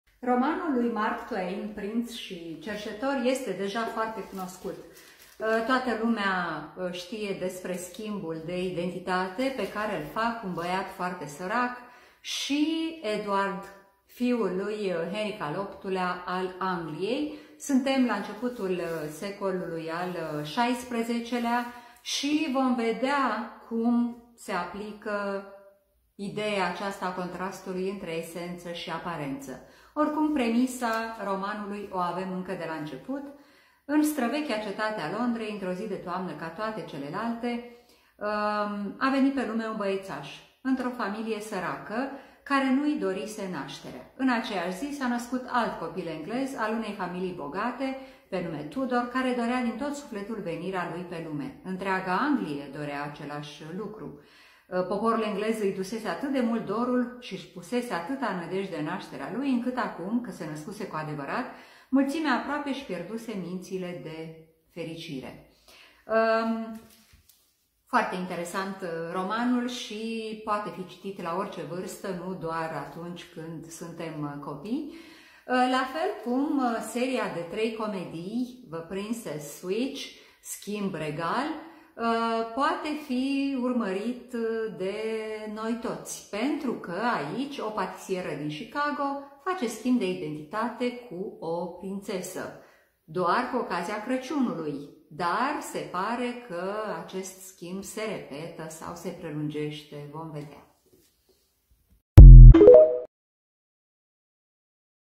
"Prinț și cerșetor", de Mark Twain & "The princess switch"- filme
Schimb de roluri/identitate în romanul lui Mark Twain și în trei comedii, "Un schimb regal"